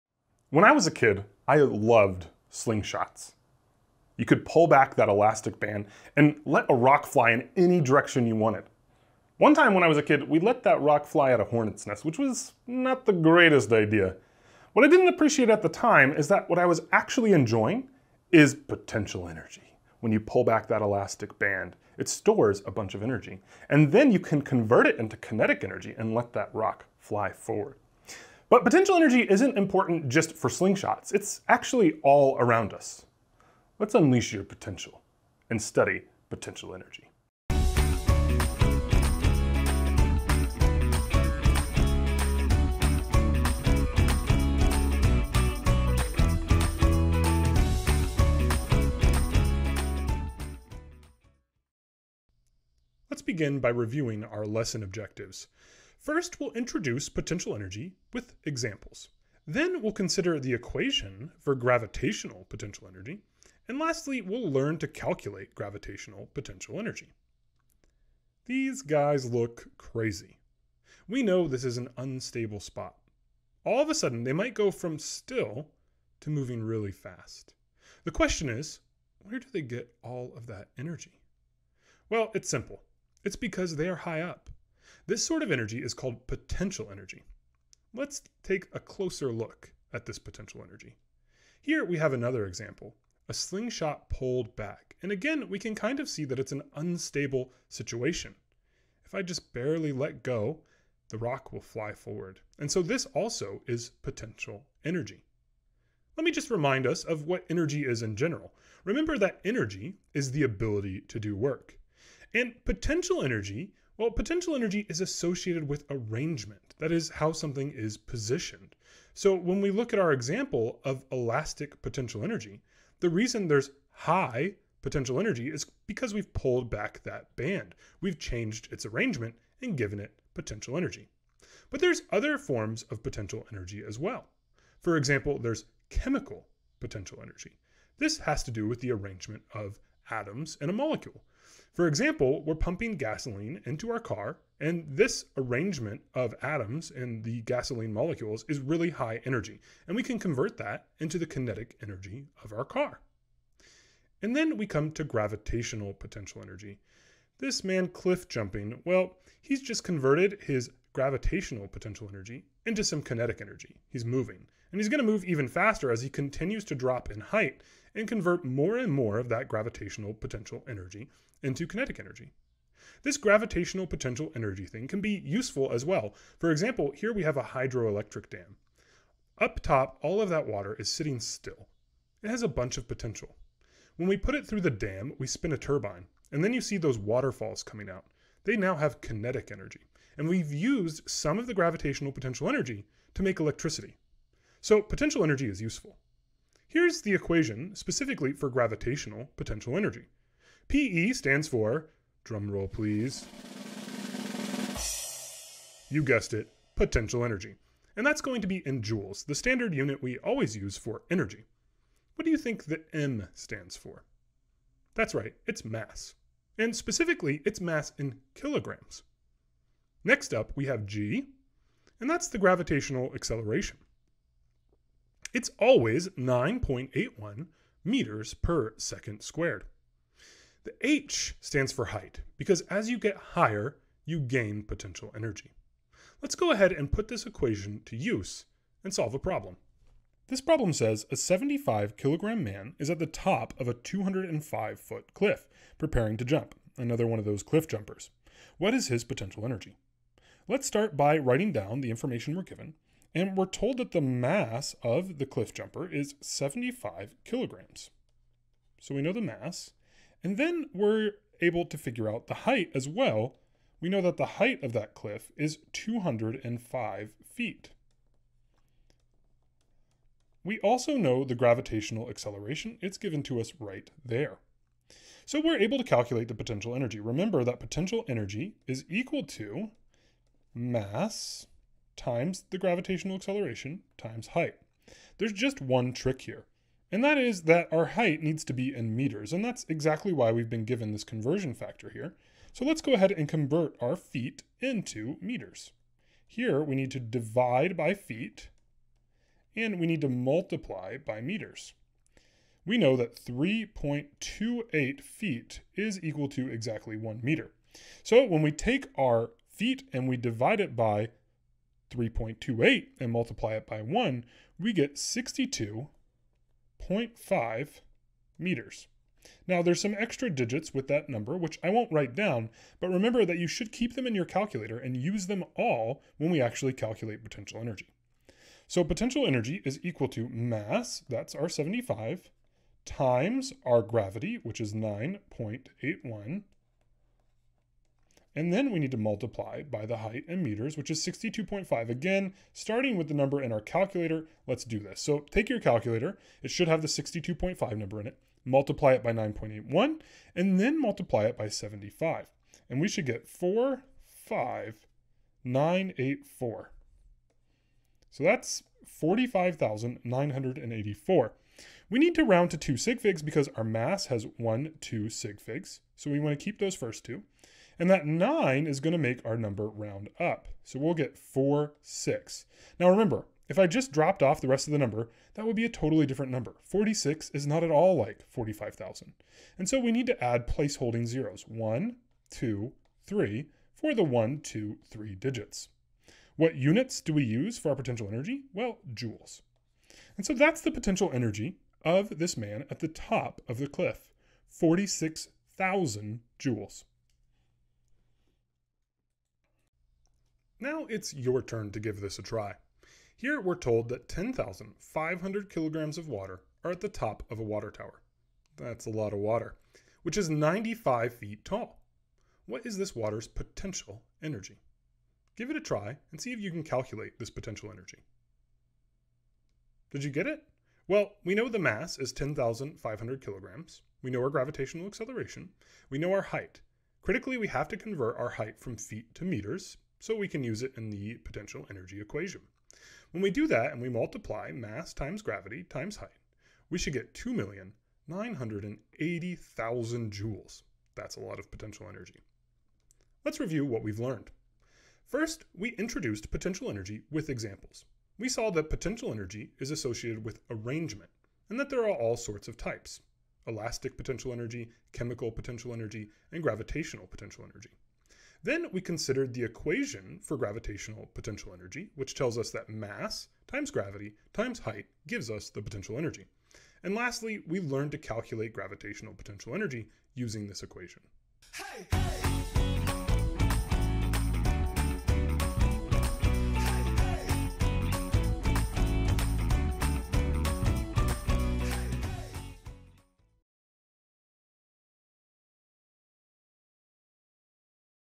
Potential Energy - Calculating Gravitational Potential Energy
In this high school physics lesson, students will learn how to recognize and calculate the gravitational potential energy of elevated objects. This lesson is part of MiaPrep’s Physics course.